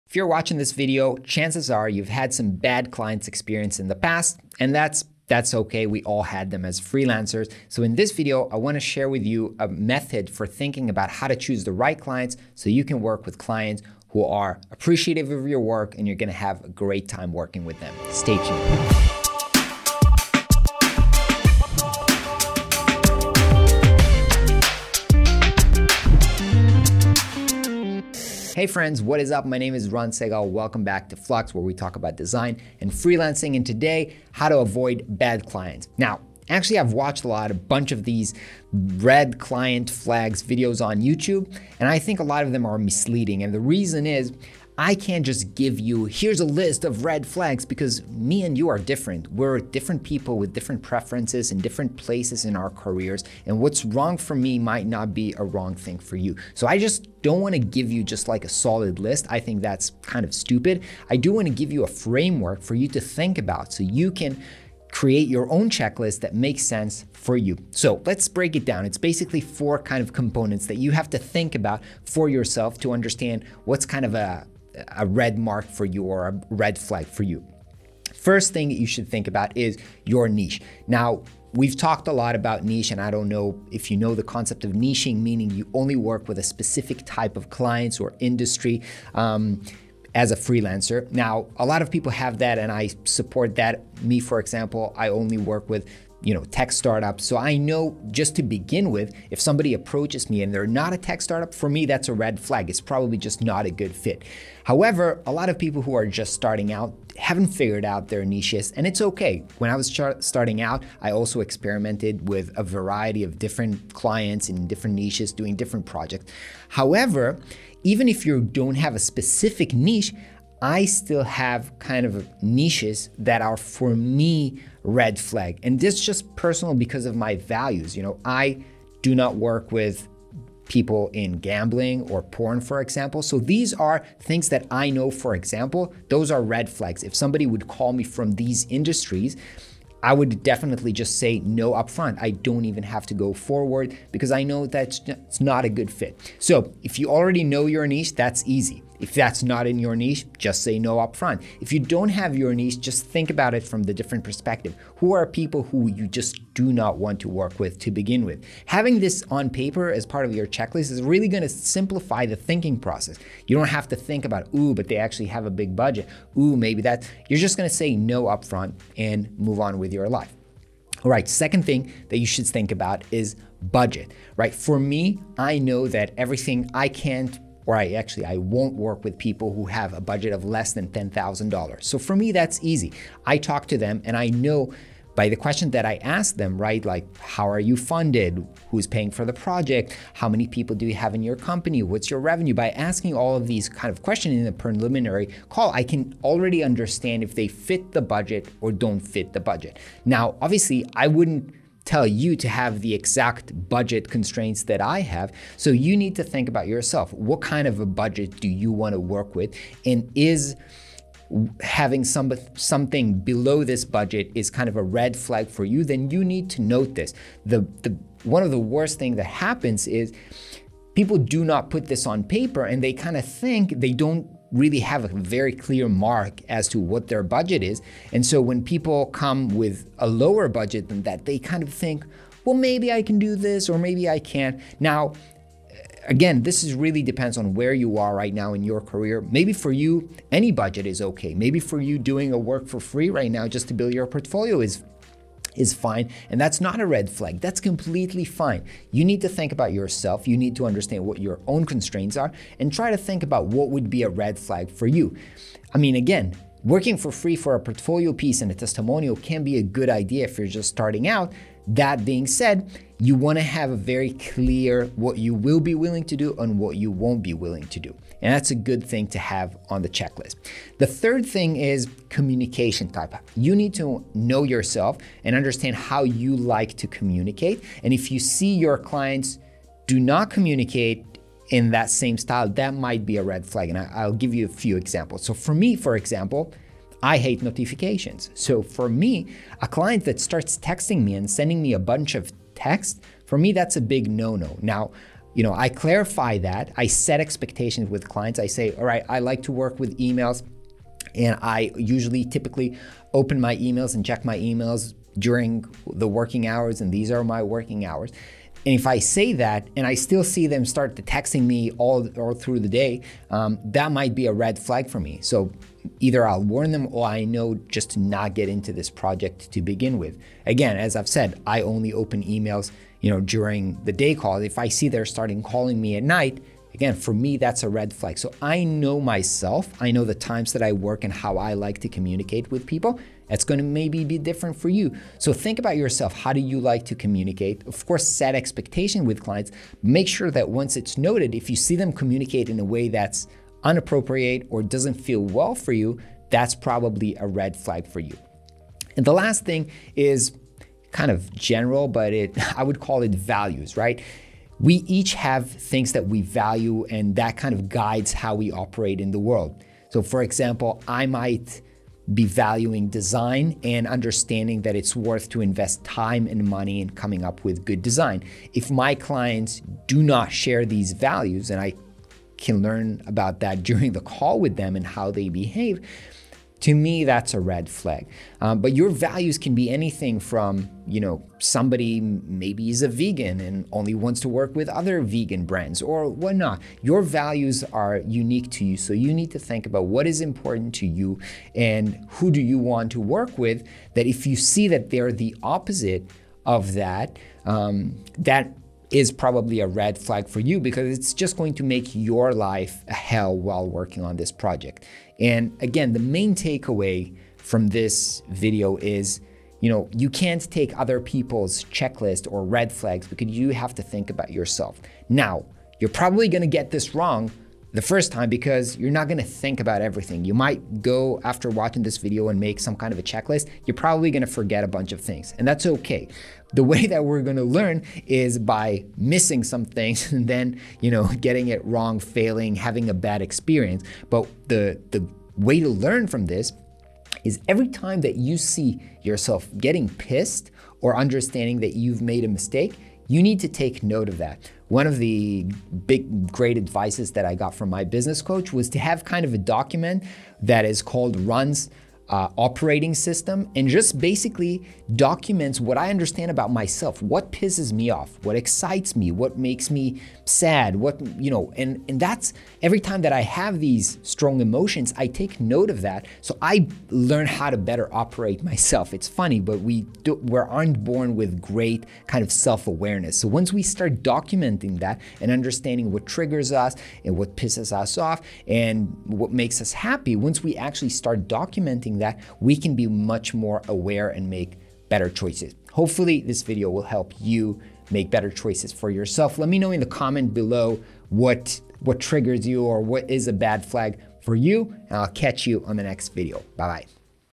Client Red Flags You Need to Be Aware Of
Client red flags vary from freelancer to freelancer. We're all in different stages of our freelance careers and all value different things, so it's only natural.

However, in this video I'd like to share a simple framework that any freelancer can use to vet clients.

Just getting started in freelance? We wrote a full guide to help get you started

Timestamps:
00:00 Intro
1:20 Niche
2:55 Budget
4:30 Communication
5:53 values